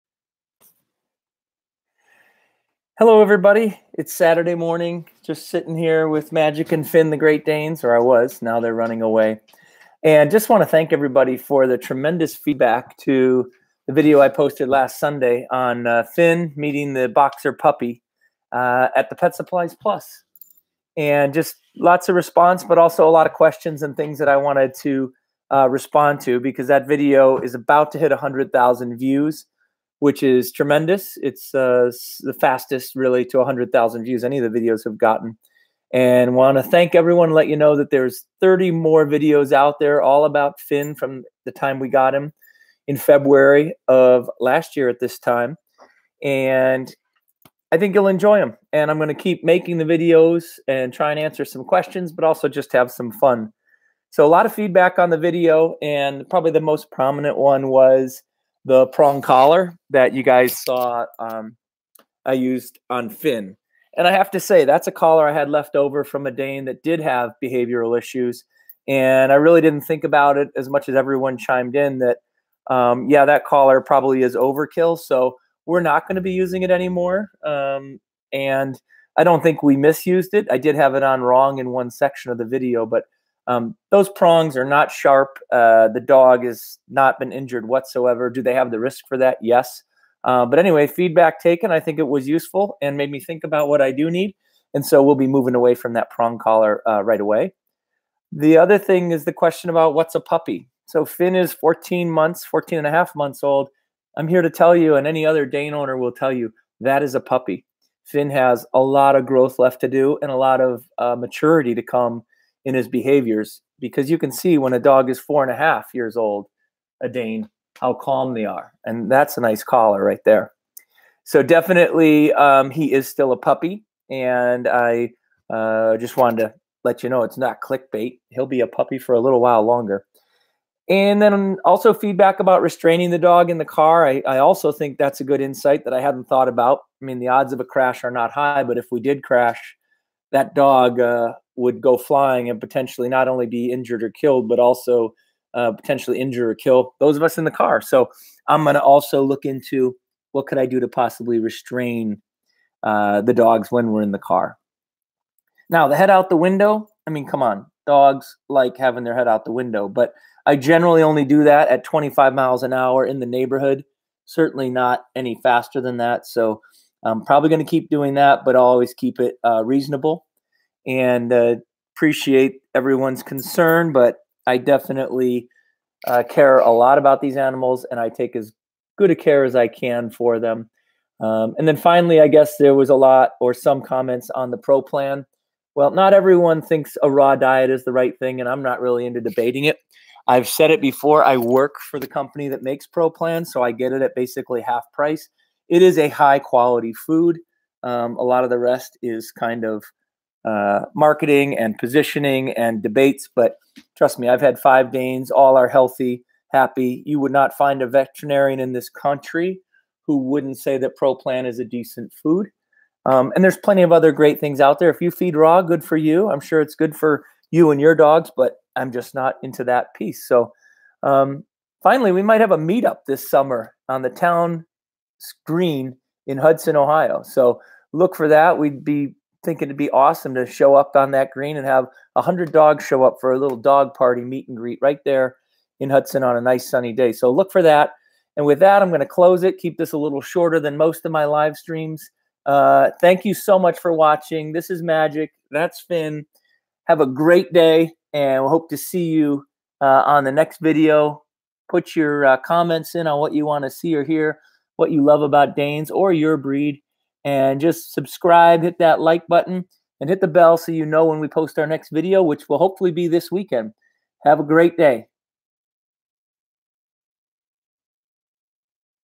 Dane Puppy meets Boxer Puppy responses to comments
When you get 165K views in 7 days, you are likely to get a lot of global feedback on your practices as a dog owner.  Some good some critical.  Thanks for all that and I have gained some insights from the engagement.  Enjoy.

I mostly make videos about my two Great Danes.  I hope you enjoy them. Sometimes I make other vlogs which I hope you can tolerate.